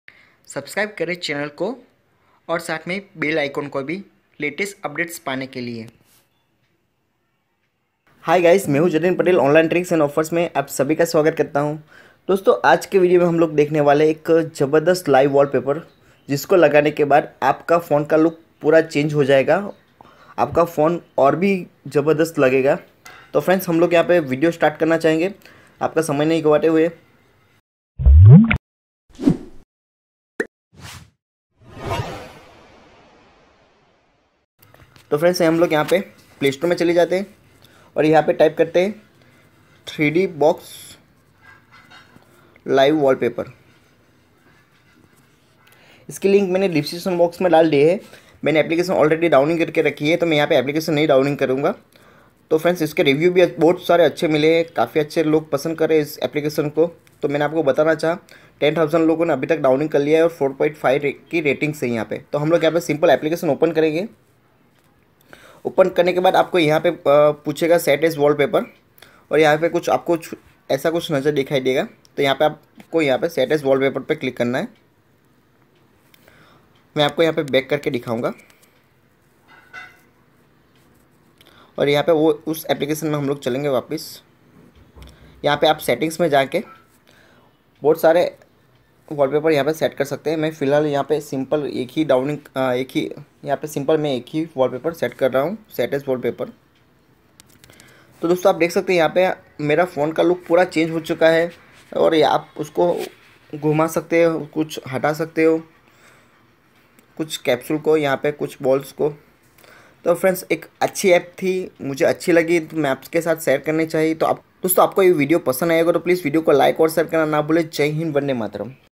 Best Phone Backgrounds aur 3D Live wallpaper kaise Rakhe Apne Phone Me.
hello friends.
is video me aap dekhoge 3D Live wallpaper kaise rakhe apne phone me.